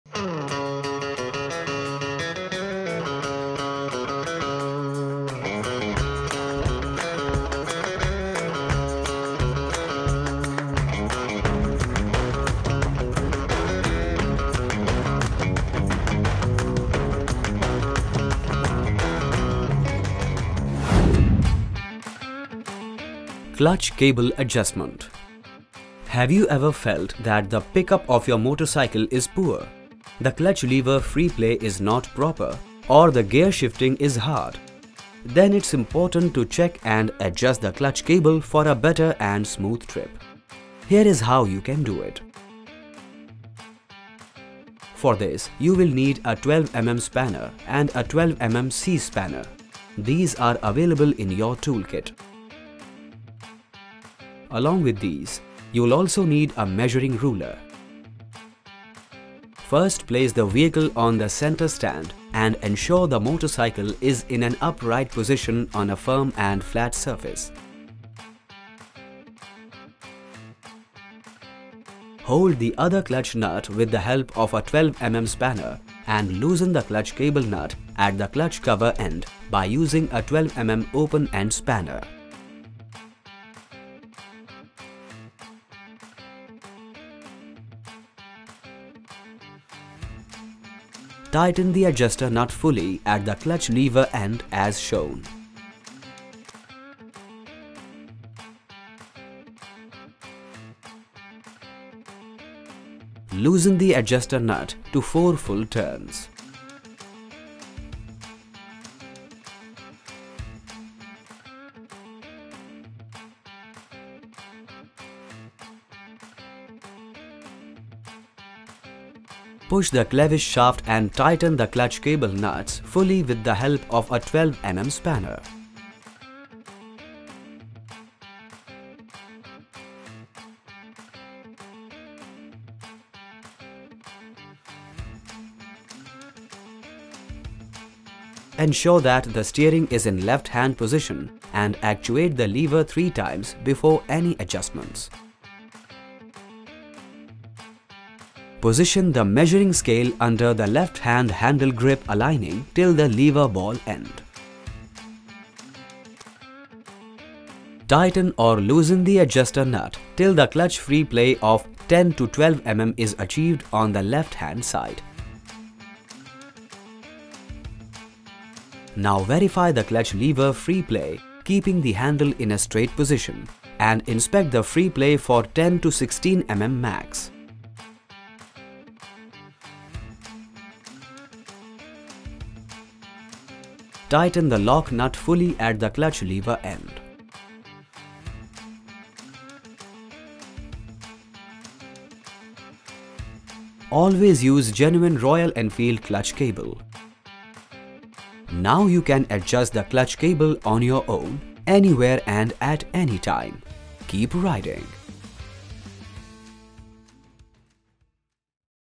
Clutch Cable Adjustment | Hunter 350 DIY
Facing problems with the pickup, gear transmission, or the clutch  free play of your motorcycle? Your clutch cable might need some adjusting. Here’s how to adjust the  clutch cable of your Hunter for a smoother ride to your next destination.